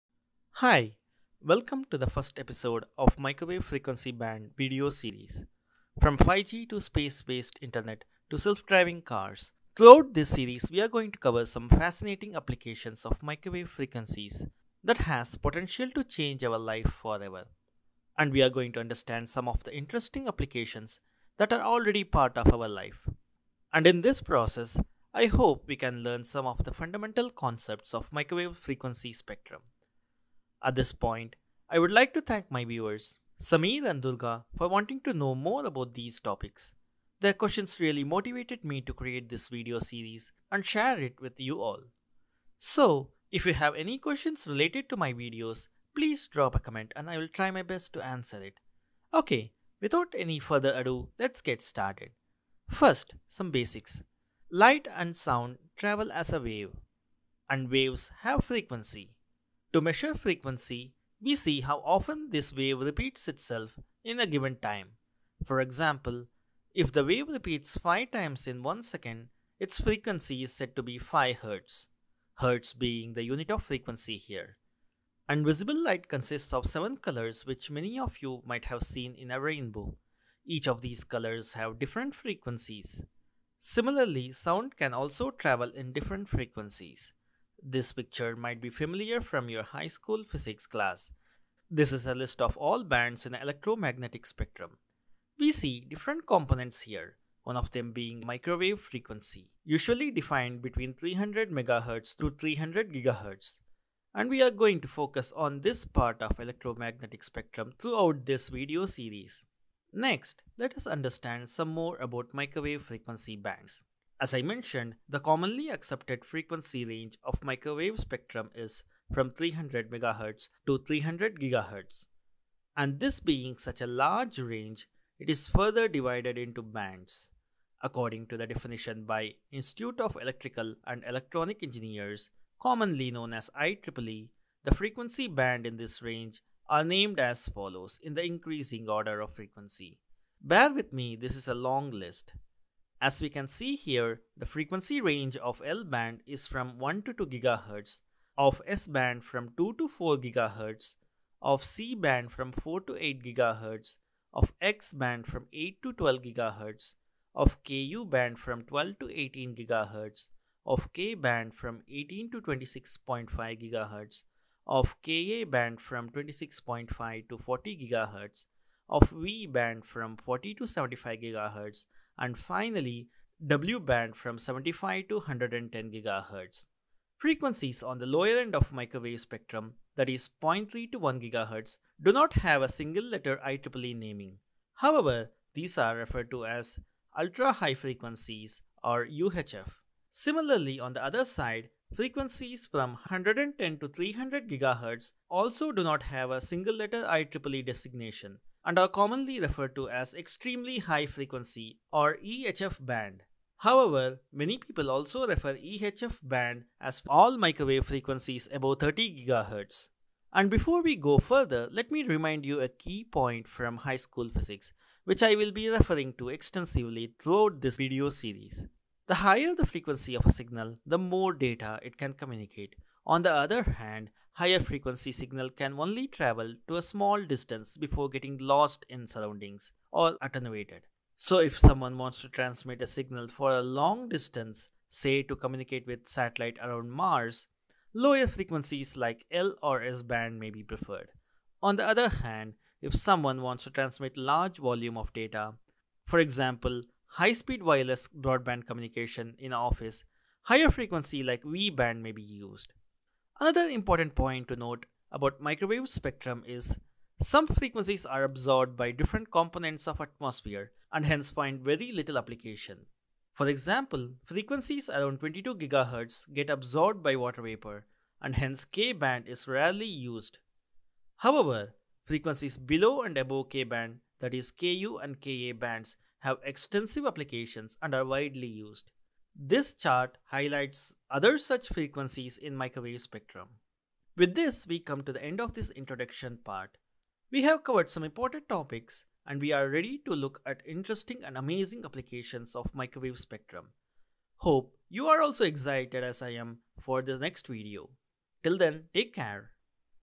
MW P1: Introduction to Microwave frequency bands
In the first part of video series about microwave frequency band, we are going to see the basics of microwave frequency.

In this series,  we will look at some interesting and fascinating applications of MW frequency band. In the process we will also try to understand  Fundamentals of MW frequency spectrum.